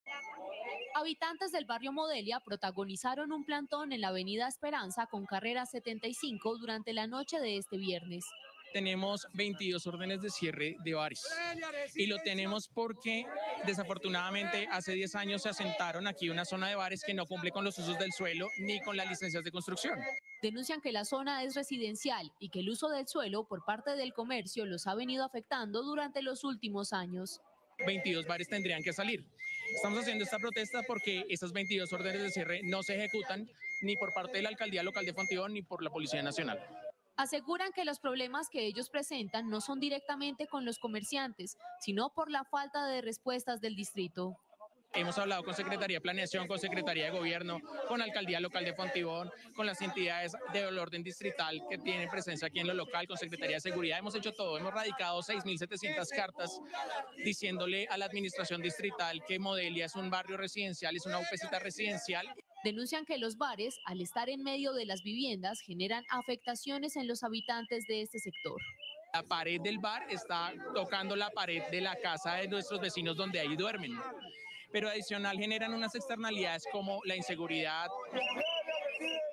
Habitantes de Modelia denuncian inconformidad por bares | El Tiempo
Habitantes de Modelia realizaron un plantón en la Avenida de la Esperanza con Carrera 75. Denuncian lo que para ellos es el mal uso del suelo debido a los bares que se ubicaron en la zona residencial, piden la intervención del distrito.